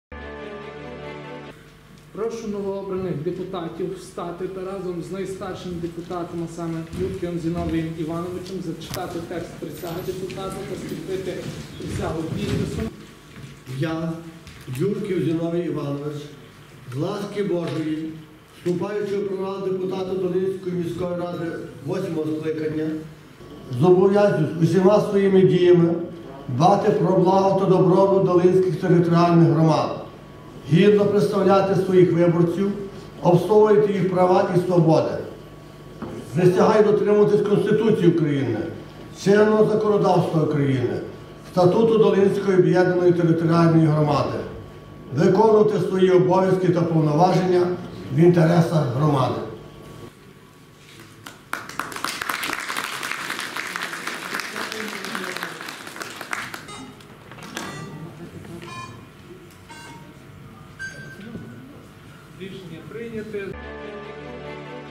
Новообрані депутати восьмого скликання склали присягу на урочистому засіданні міської Ради
Новообрані депутати восьмого скликання склали присягу на урочистому засіданні міської Ради Долинської ОТГ у вівторок, 1 грудня 2020р. Текст присяги зачитав найстарший обраний депутат, Зіновій Юрків.